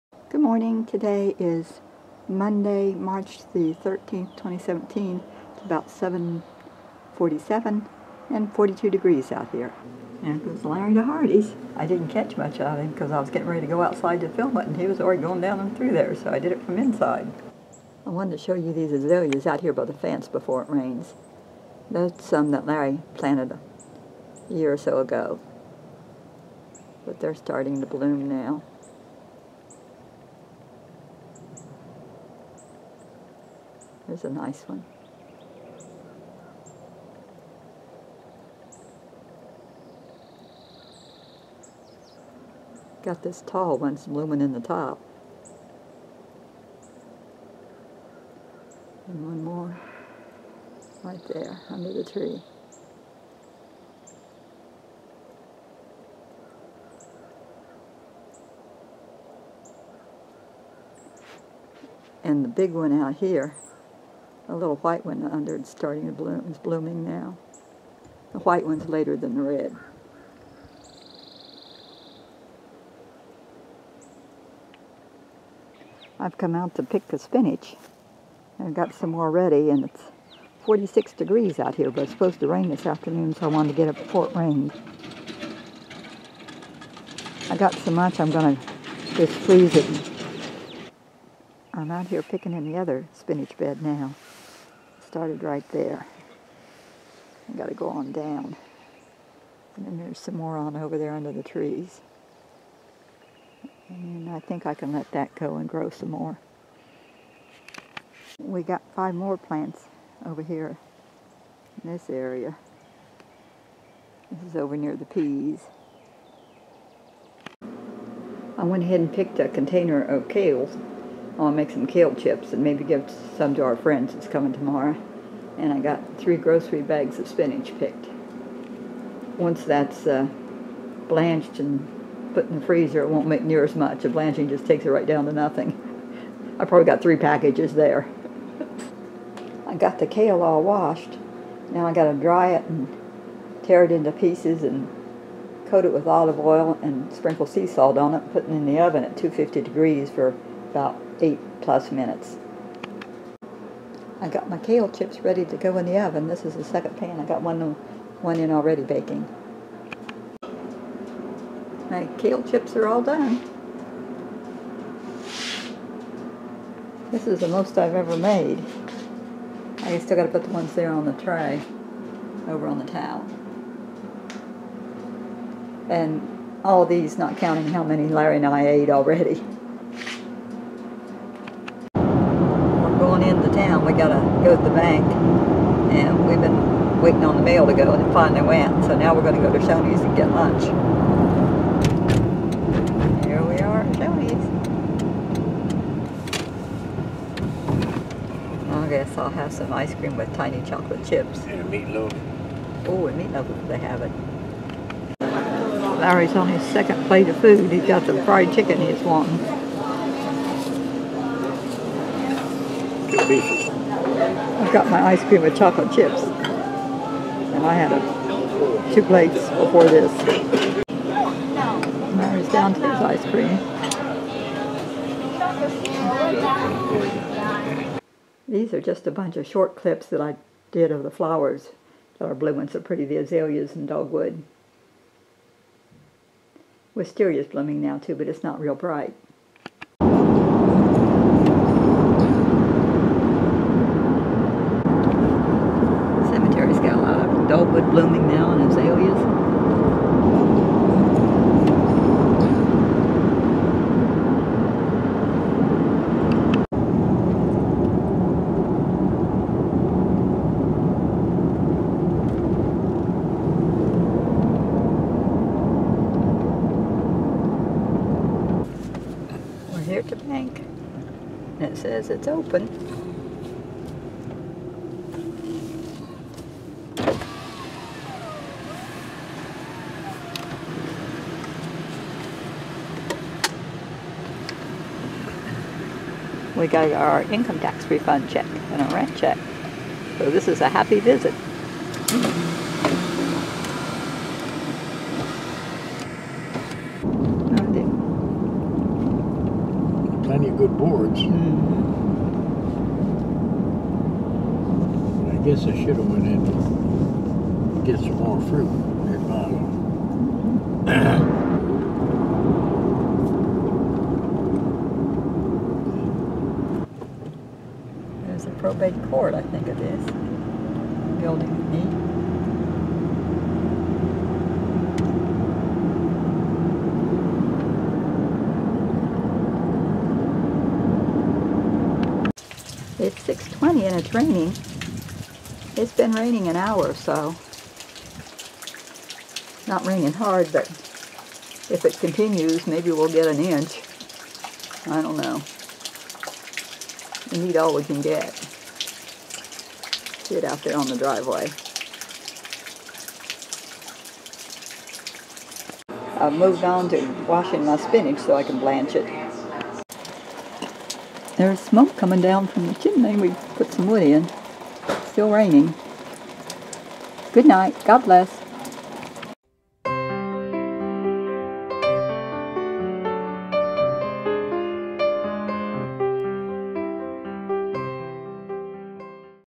20170313 Vlog - Azaleas in Bloom Plus Kale Chips and Spinach
When Larry got home from Hardee's, I picked spinach and made kale chips.  We went to Shoney's for lunch, then to the bank.  The azaleas, dogwood, and wisteria are in bloom everywhere.  We got some rain.